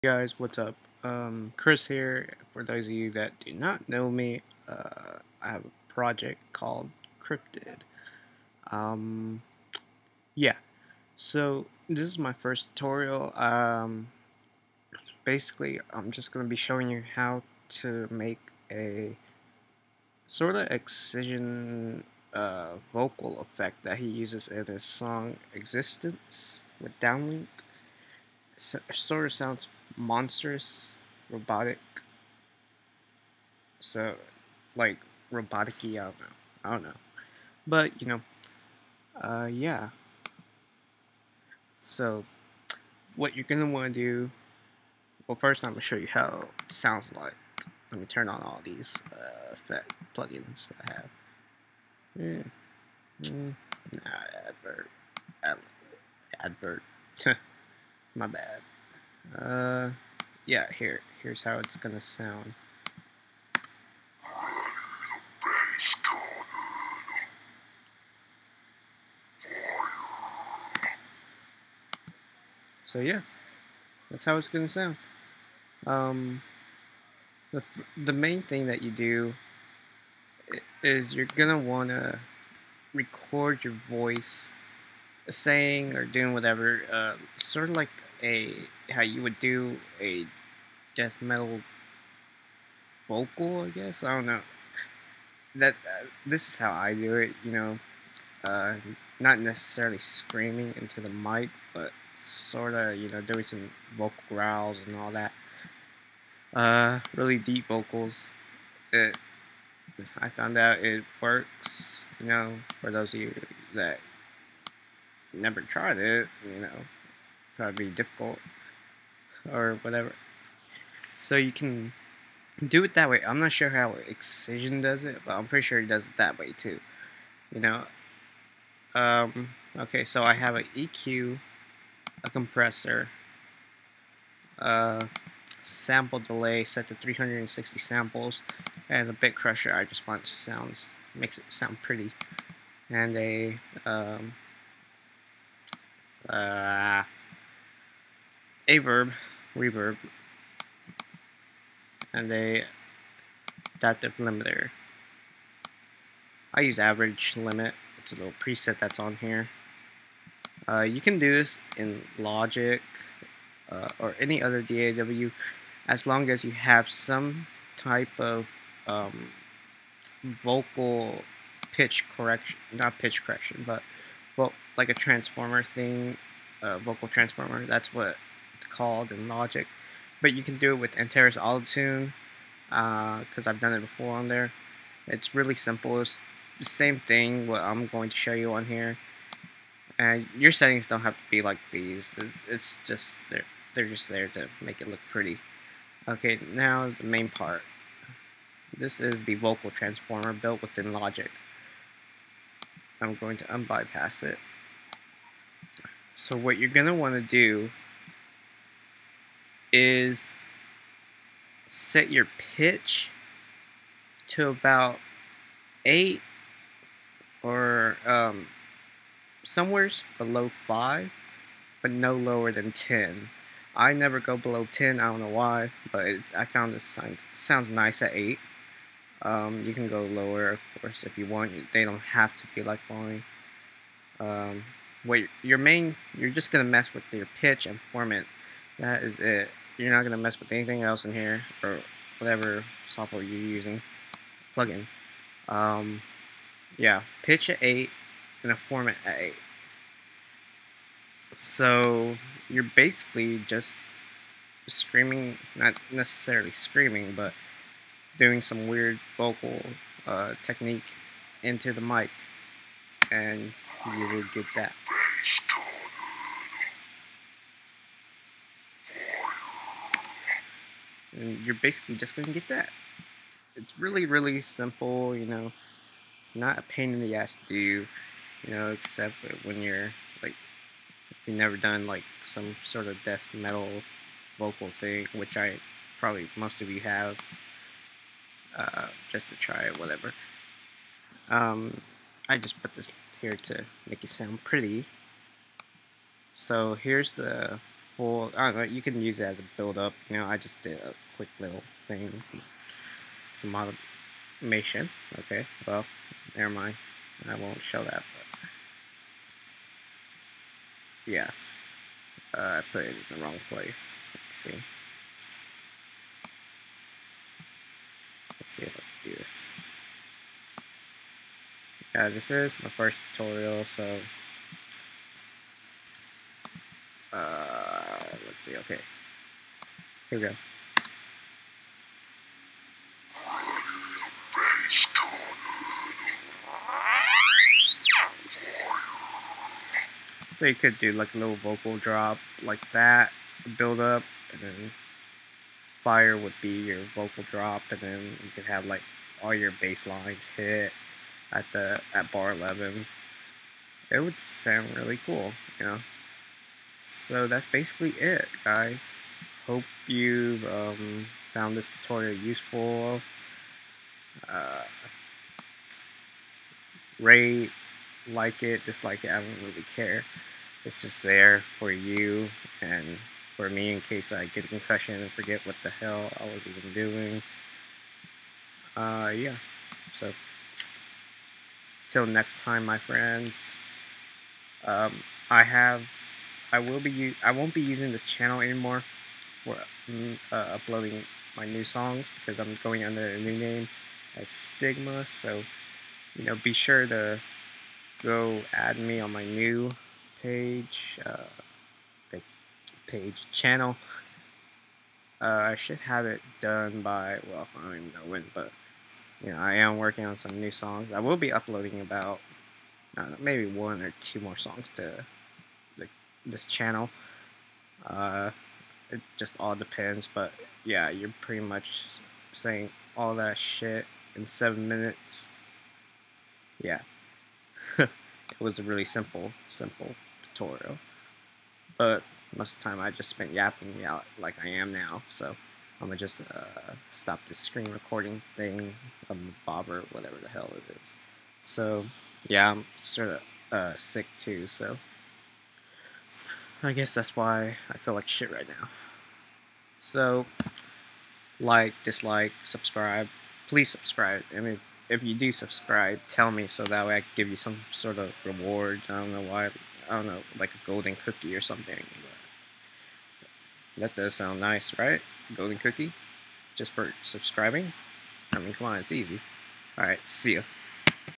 How to make an Excision vocal effect in Logic Pro BRO! (or any other DAW)
my first tutorial in Logic Pro. How to make your voice sound like Excision in logic or ANY other DAW with a vocal transformer or pitch plug in. :3 LIKE me on facebook!

Dont like the tutorial? fuck off then. :]